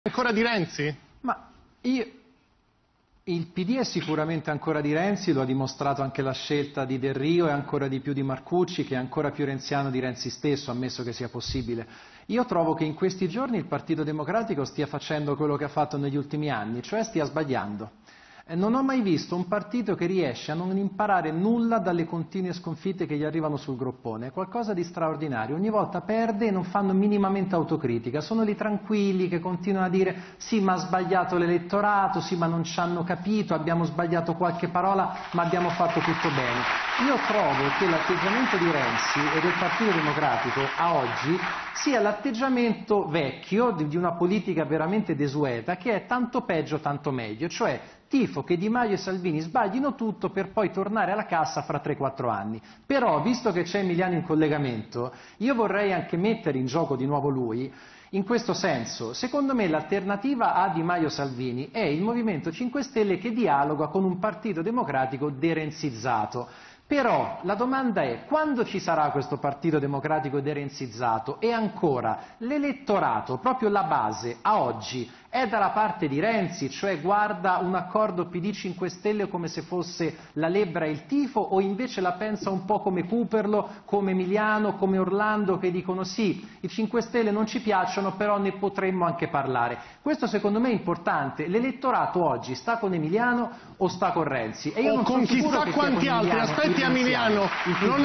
Andrea Scanzi: 'Il PD è ancora di Renzi, bisognerebbe de-renzizzarlo'
Andrea Scanzi (Il Fatto Quotidiano): 'Il Partito Democratico sta sbagliando, non riesce ad imparare nulla dalle sconfitte'.